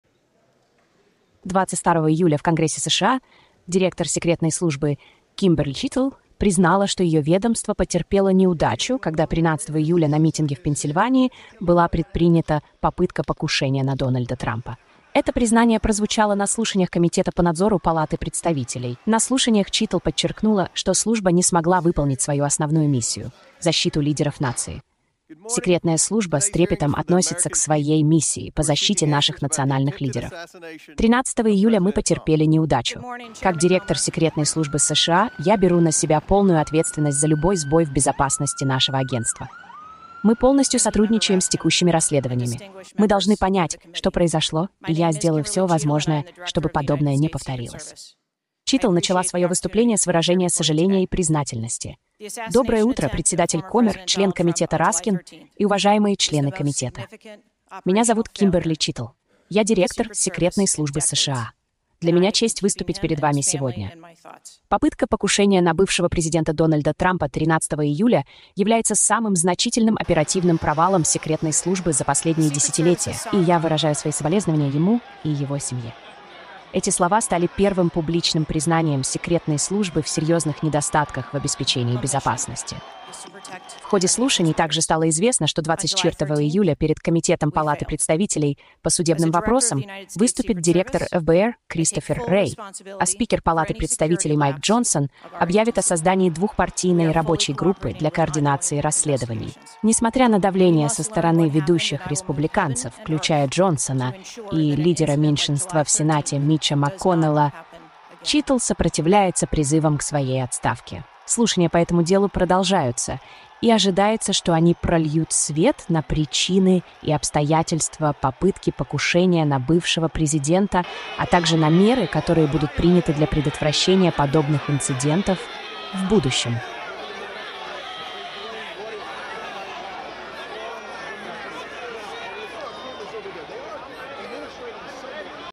Трамп - исторический провал секретной службы США!
Глава секретной службы США на слушаниях в конгрессе призналась, что стрельба в президента США небывалый, исторический провал сотрудников спецслужб